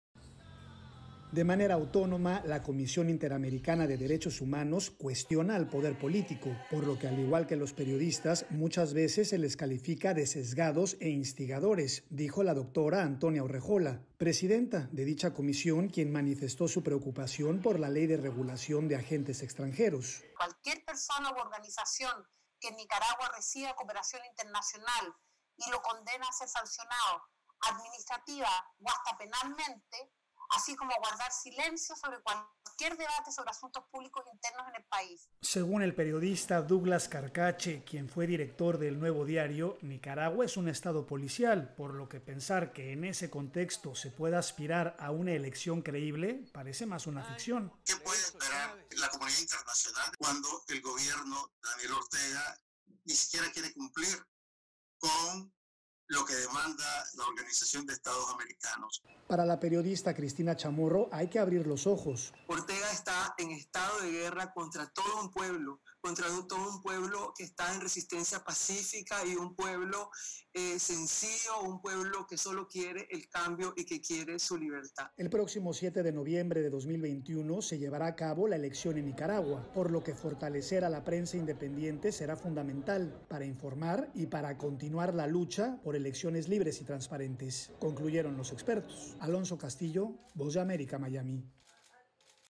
Opresión Gubernamental en Nicaragua
La opresión gubernamental en Nicaragua fue el último seminario que abordó la Sociedad Interamericana de Prensa, SIP. En poco más de medio año en ese país se celebrarán elecciones para elegir desde el Presidente hasta los diputados. Un proceso electoral que se llevará a cabo, según dijo una de las panelistas, en un estado de guerra. Informa Alonso Castillo, de la Voz de América, en Miami.

¡Visítenos!